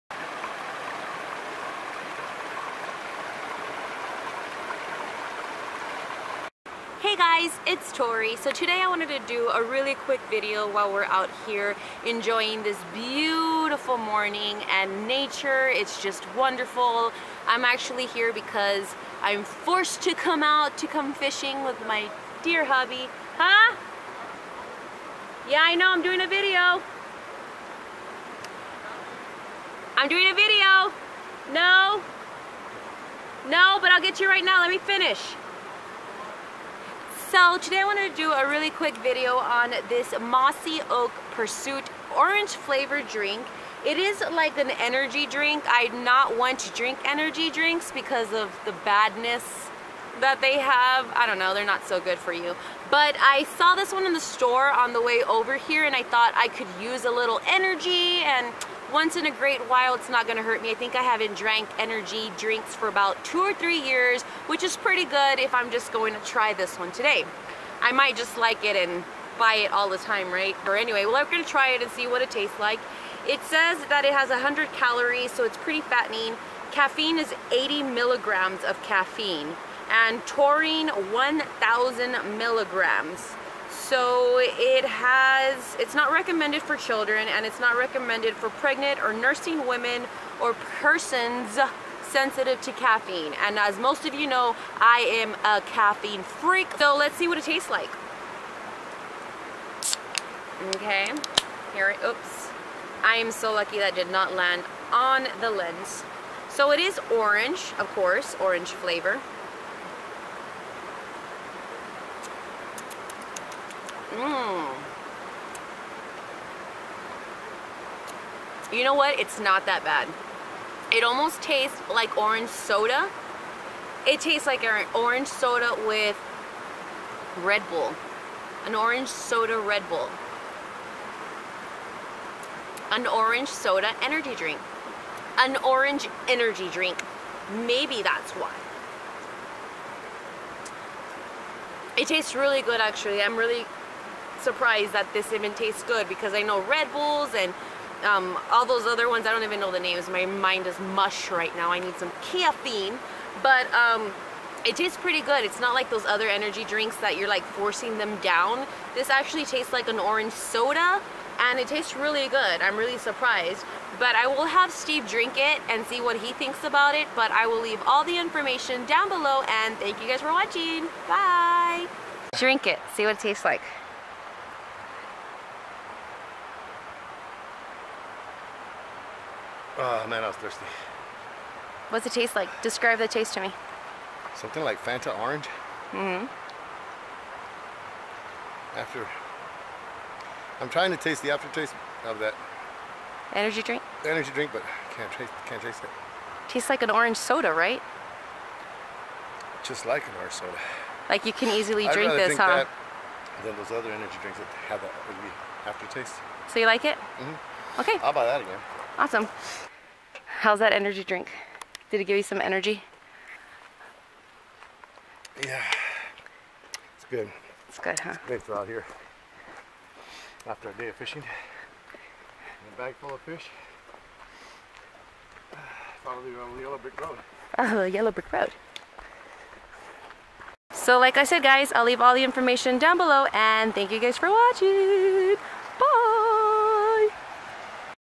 Mossy Oak Pursuit Energy Orange
Wow, I am so surprised how great this energy drink taste! It actually doesn't even taste much like an energy drink , it taste more like just a good 'ol orange soda, I'm not kidding either, it's very hard to taste the "energy drink" part in this drink. t has a great orange soda flavor and seemed to give us some pretty cool energy, we are not one's for drinking energy drinks but I saw this at our local grocery store and it caught my attention, so I thought I'd do a first impression video, that and because I didn't have my coffee before we left home that morning. Not sure how healthy it is for you as an everyday drink but for a once in a while , it taste great for us.

Pros:
Gives Energy
Taste Great

Cons:
Not on the healthy side